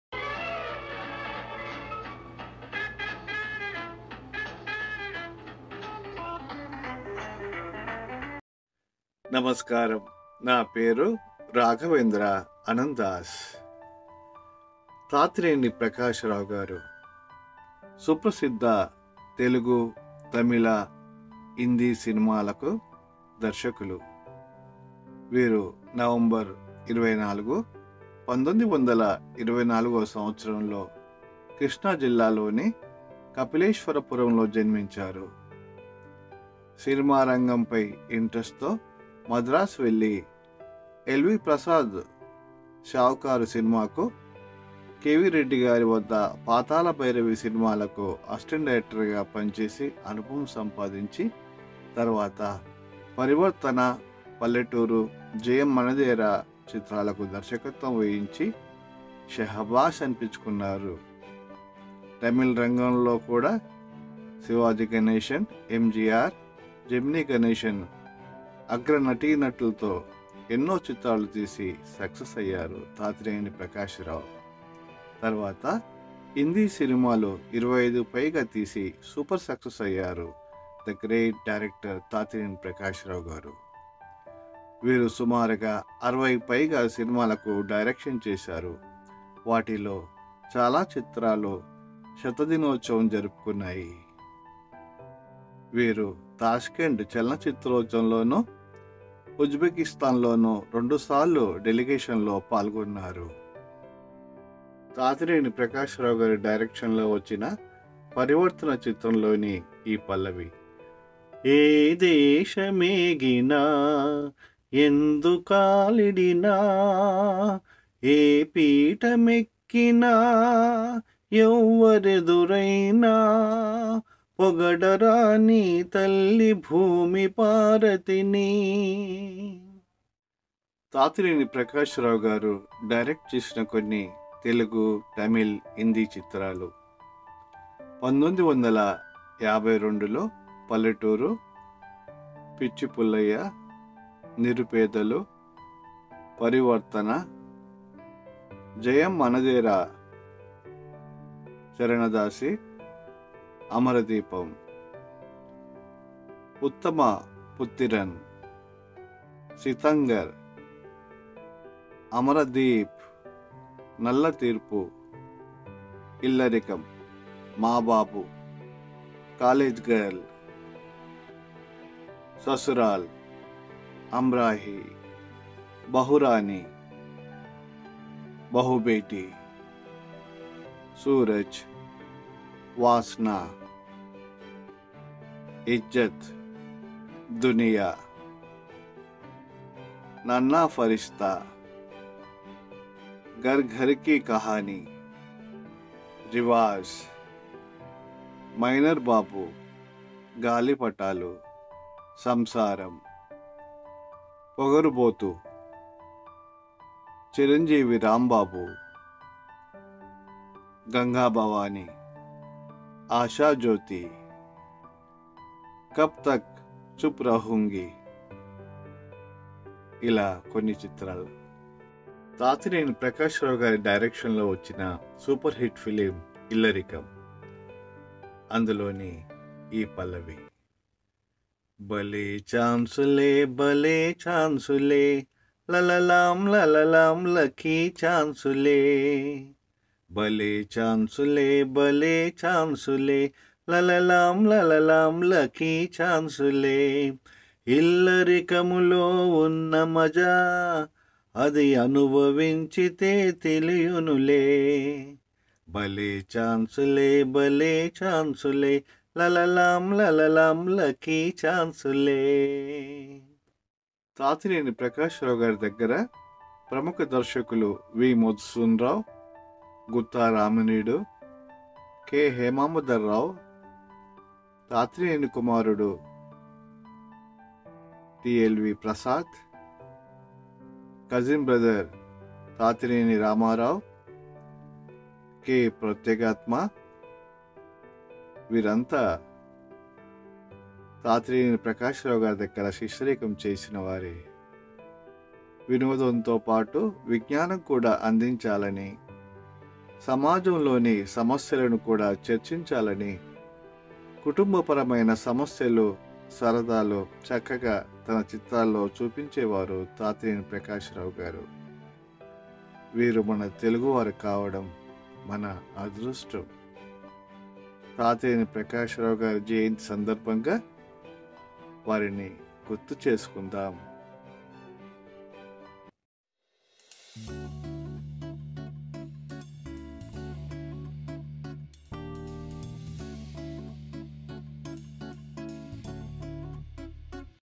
Senior Director Tatineni Prakash Rao gari Jayanthi 🙏 Host by Raghavendra Anandas
Tatineni Prakash Rao was an Indian filmmaker. He made a number films in Telugu, Tamil and Hindi languages. His first film was palletooru. He was assistant director in Shavukar and later directed Pitchi Pullaiah, Jayam manade starring N. T. Rama Rao. He worked with both NTR and ANR in films CharanaDaasi and Parivarthana.